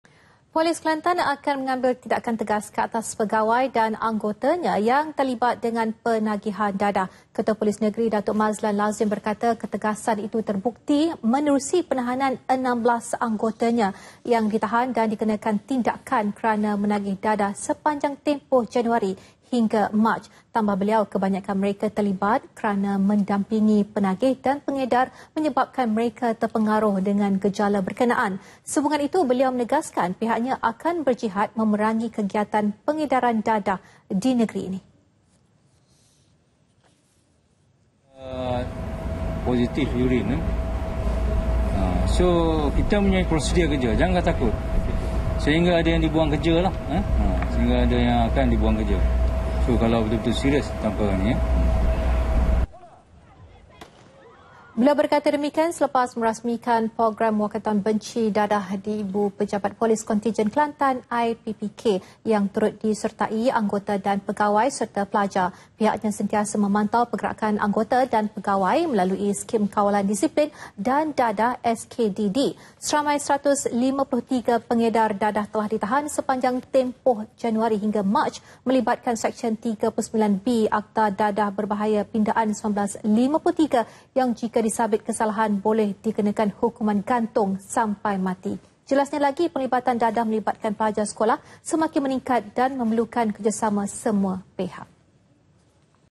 POLIS KELANTAN AKAN AMBIL TINDAKAN PADA ANGGOTANYA YANG TERLIBAT PENAGIHAN DADAH 19 APRIL 2015
Polis Kelantan akan mengambil tindakan tegas ke atas pegawai dan anggotanya yang terlibat dengan penagihan dadah.

Ketua Polis Negeri, Datuk Mazlan Lazim berkata, ketegasan itu terbukti menerusi penahanan 16 anggotanya yang ditahan dan dikenakan tindakan kerana menagih dadah sepanjang tempoh Jan hingga Mac.